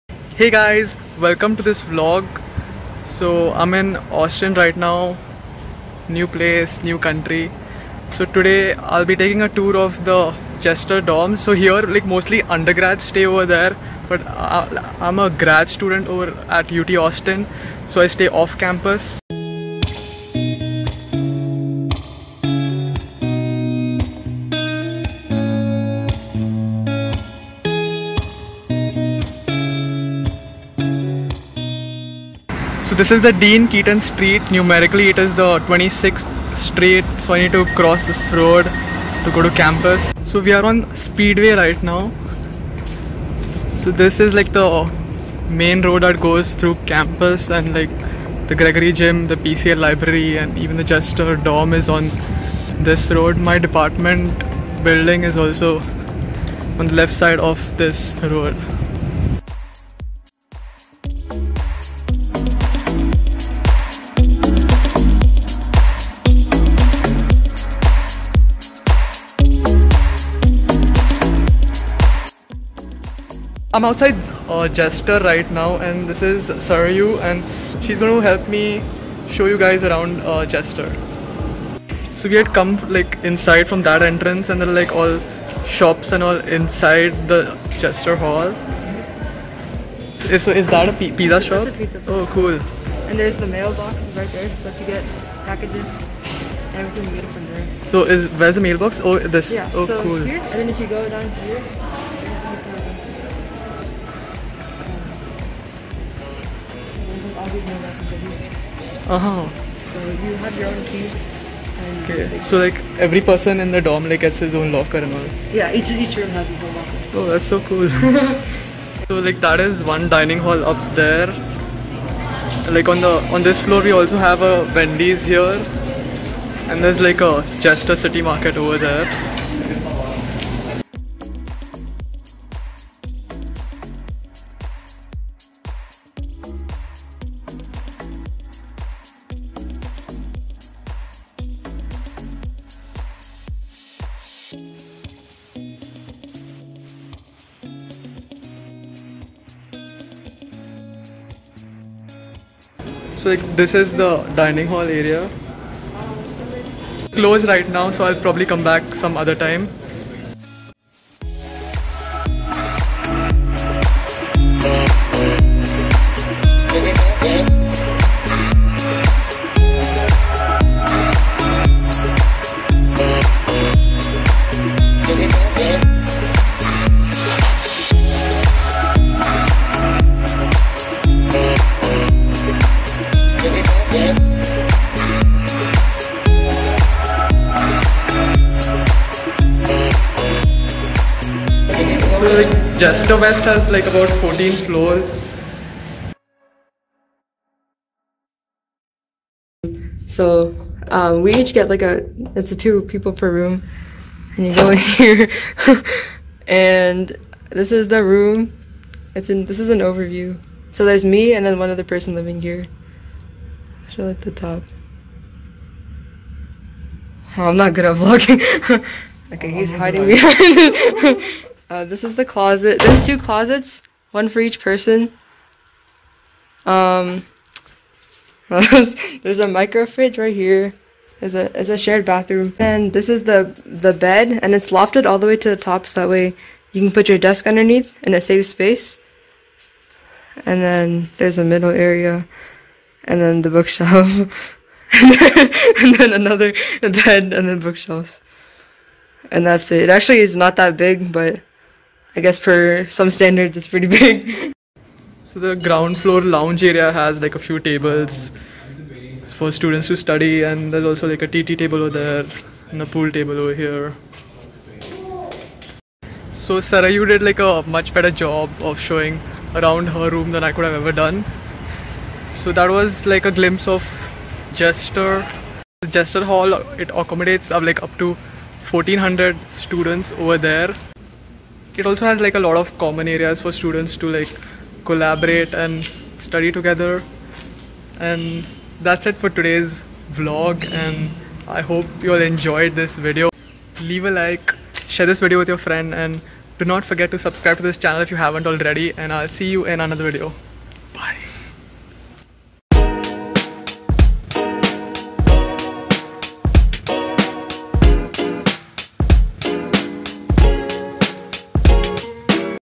COLLEGE RESIDENCE HALL VLOG | University of Texas at Austin | Vlog 1
COLLEGE RESIDENCE HALL VLOG | University of Texas at Austin

In this video I take a tour of the Jester West residence hall. Get a glimpse of a Dorm Room as well.

SEO:
UT Austin